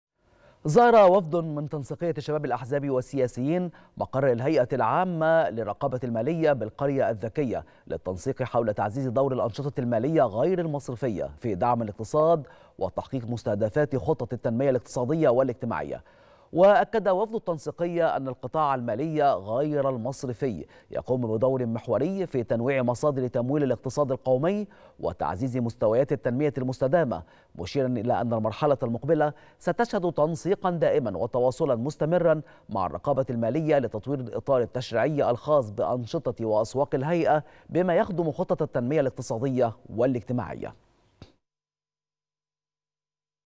وفد تنسيقية شباب الأحزاب يزور هيئة الرقابة المالية لبحث تعزيز دور الأنشطة المالية غير المصرفية
نشرة الثالثة | وفد تنسيقية شباب الأحزاب والسياسيين يزور هيئة الرقابة المالية لبحث تعزيز دور الأنشطة المالية غير المصرفية في دعم الاقتصاد